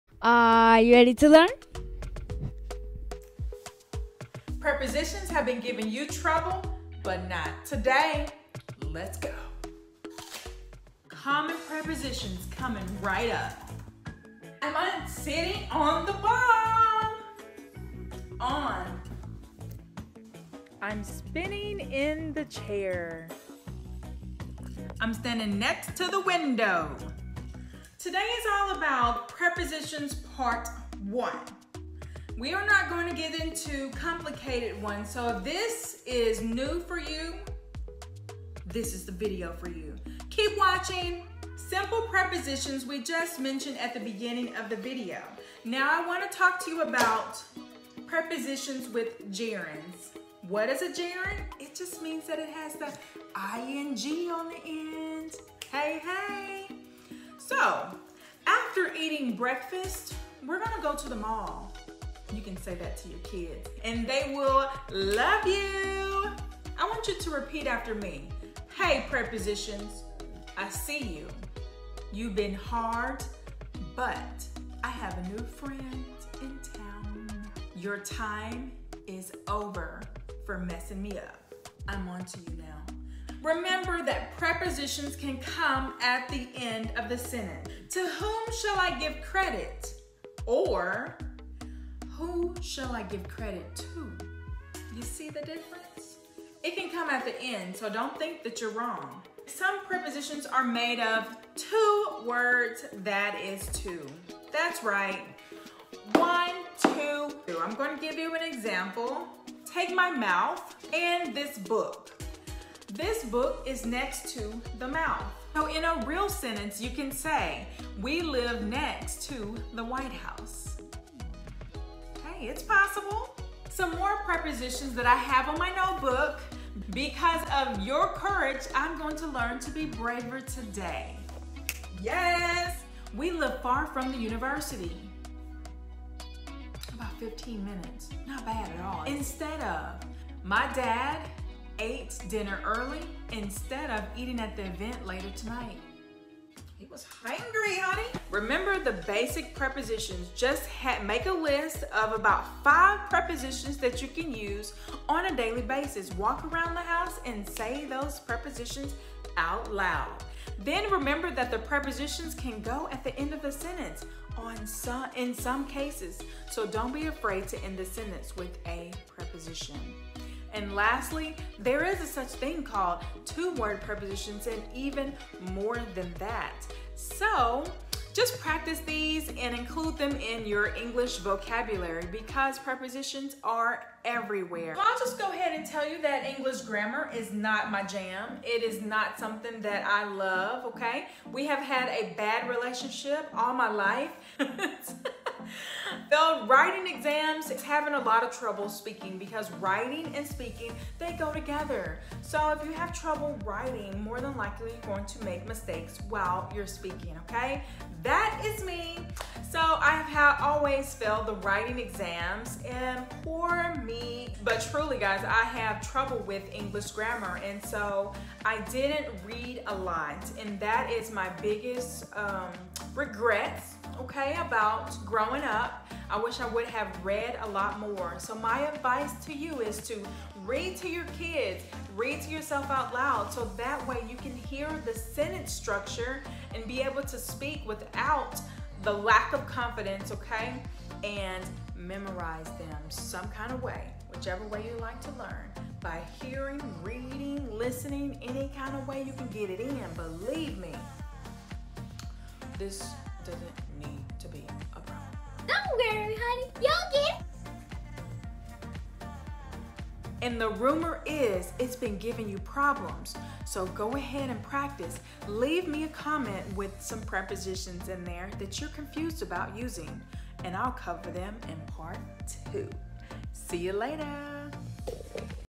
The rumor is you need help with prepositions (with LaDoris)
The rumor is english prepositions are giving you trouble.  Don't worry, I have to think about some myself and English is my first language.
If this is new for you, I didn't want to overwhelm you with a long list of them so I am giving you a quick list to start using today.

Please leave a comment to let me know what you have done to get to the level you are on🙌🏾🙌🏾

Cheers to becoming fluent,

LaDoris